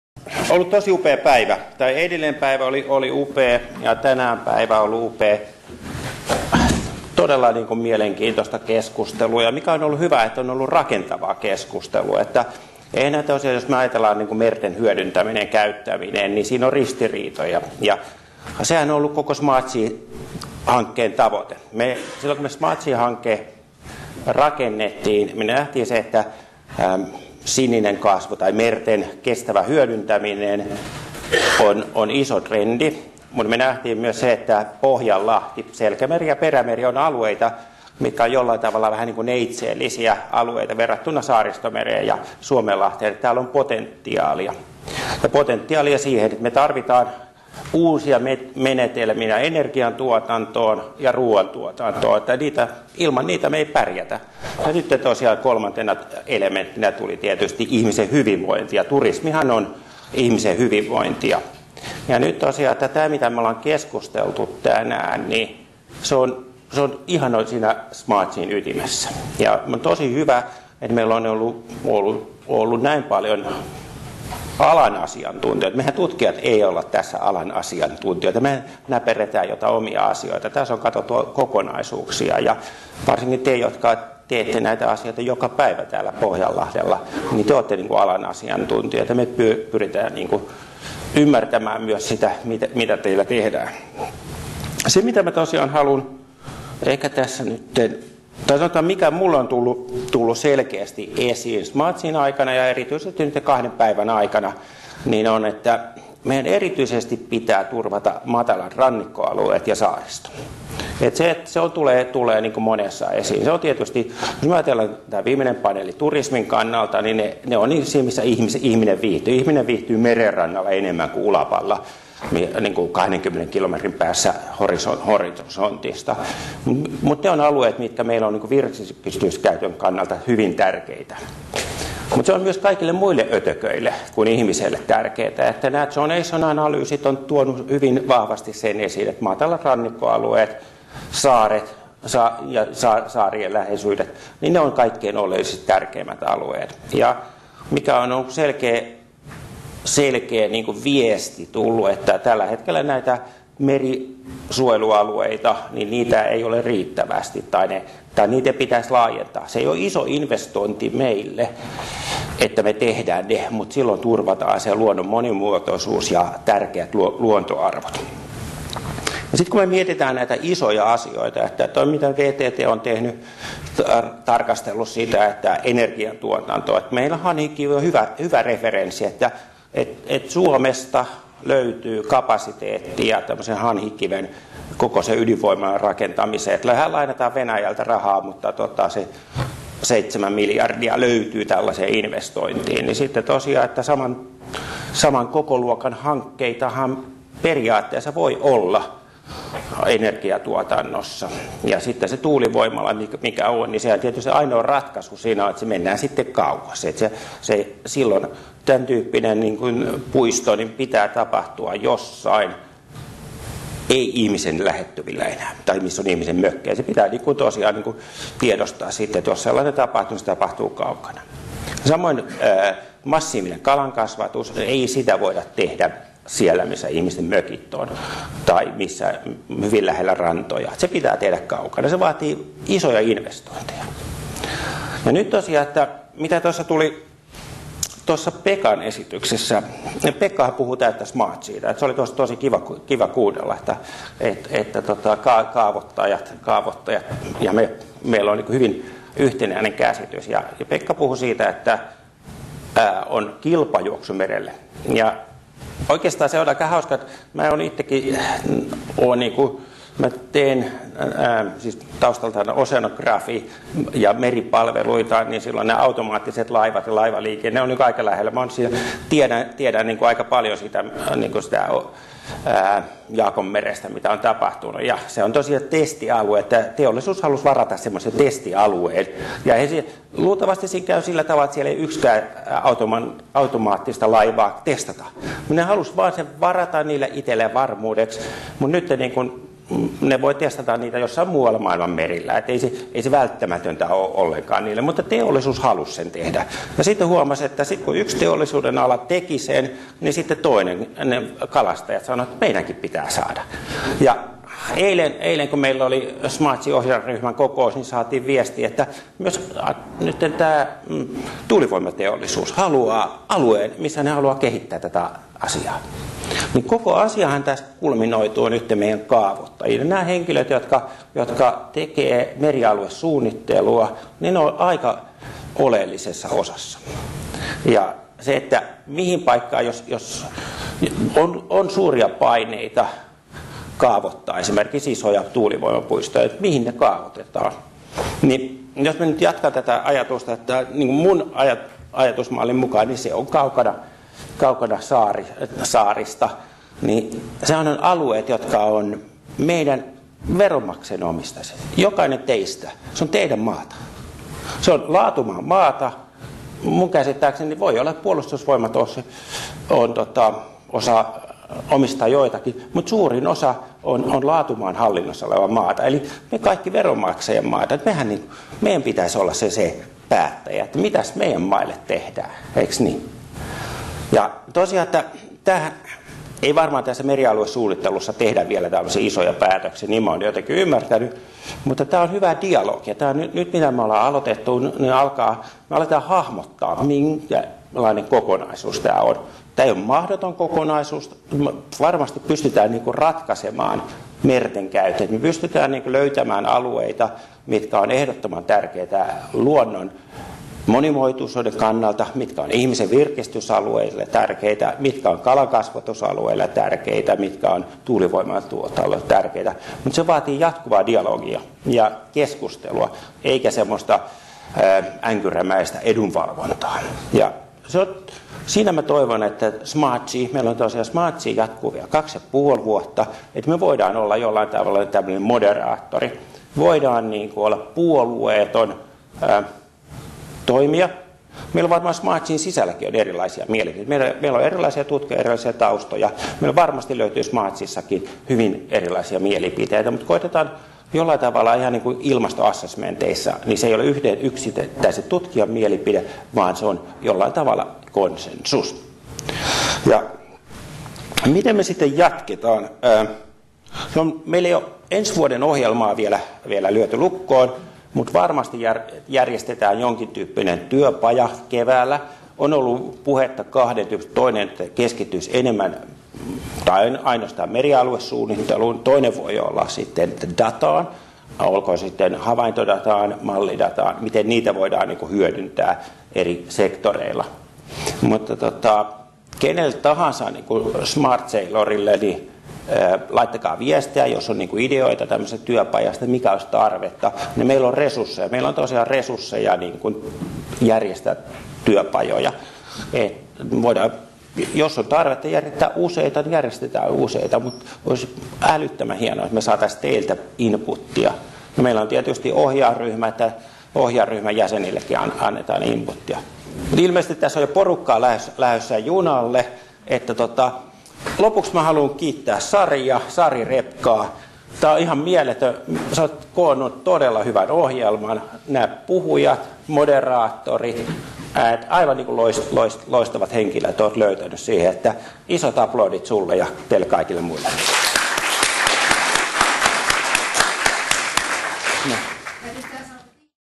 Loppuyhteenveto - Jari Haapala (IL)
SmartSea seminar Future of the Gulf of Bothnia, 18th-19th September 2018, Vaasa, Finland.